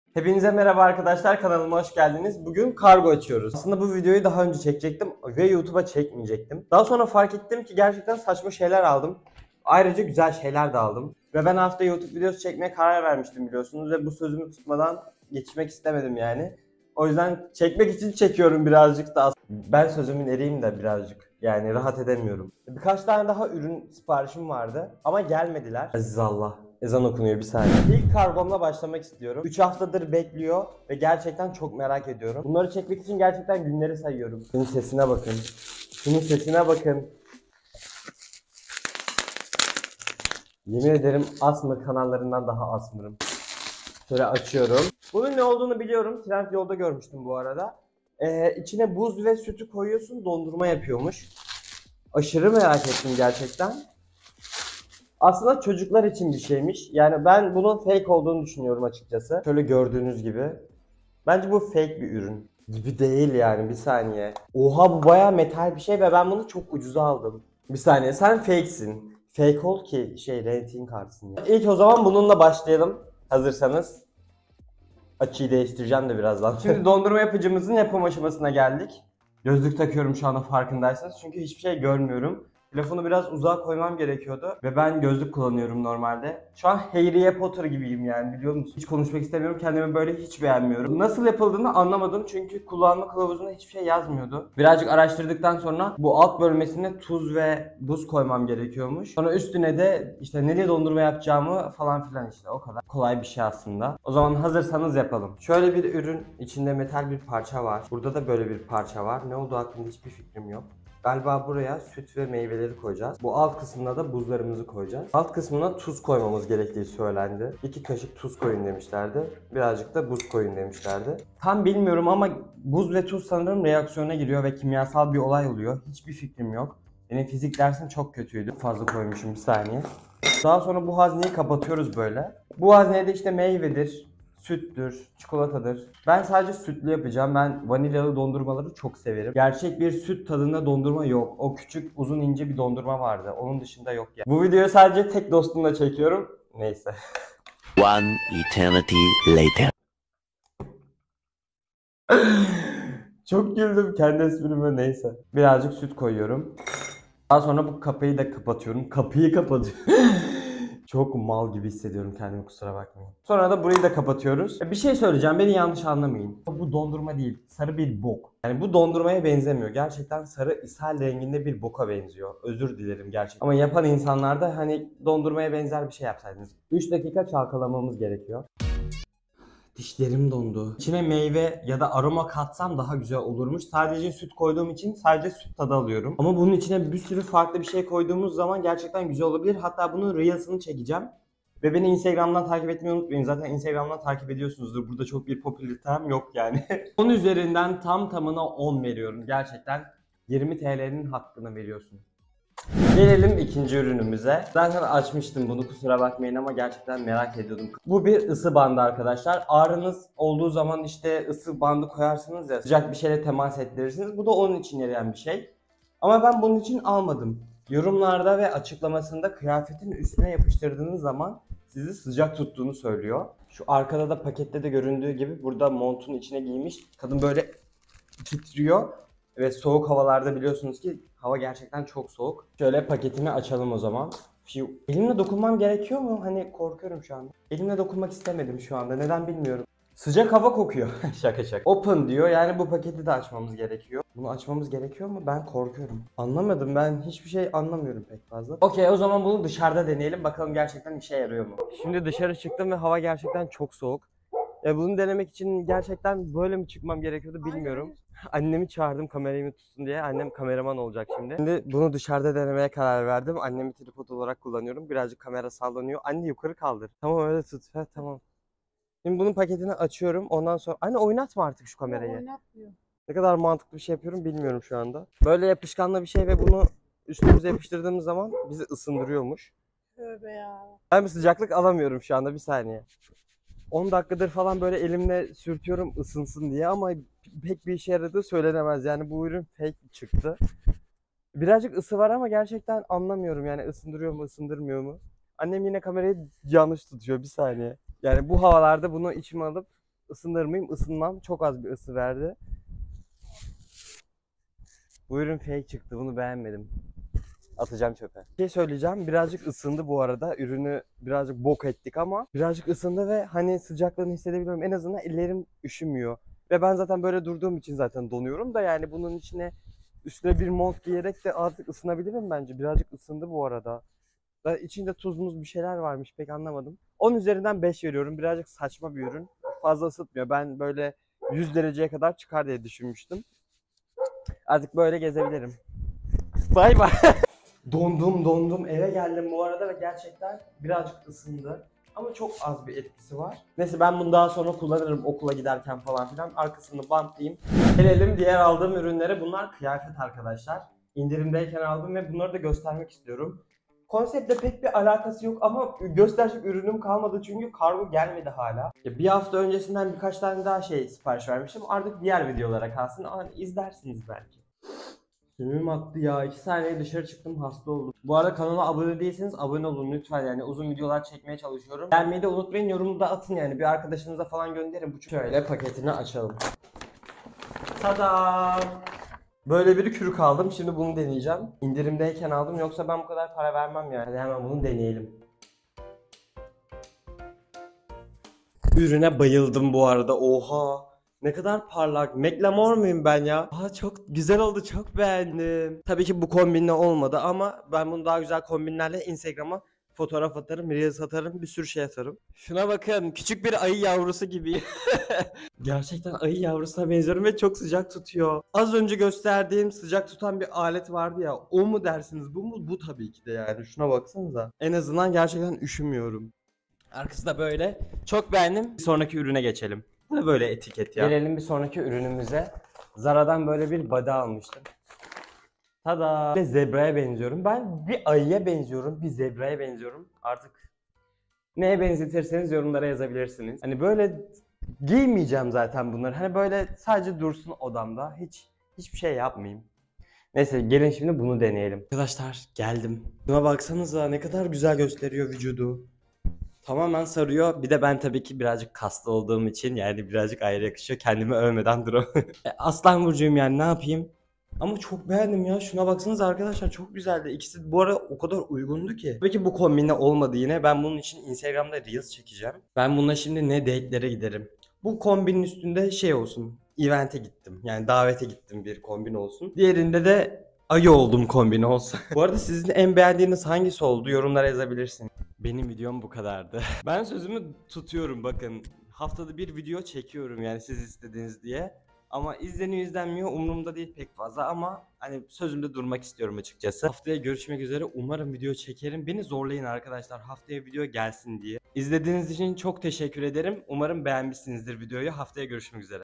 KARGO AÇILIMI - İLGİNÇ ÜRÜNLERİ DENİYORUM!
Herkese selam bugün kargo açıyorum. Birkaç paket açılımı yapmam gerekiyordu sizlerin karşısında  böyle bir video çekmek istedim. İlginç ve bir o kadar işe yarar ürünleri bu videoda sizlerle birlikte inceledik. Umarım beğenmişsinizdir hepinize iyi seyirler.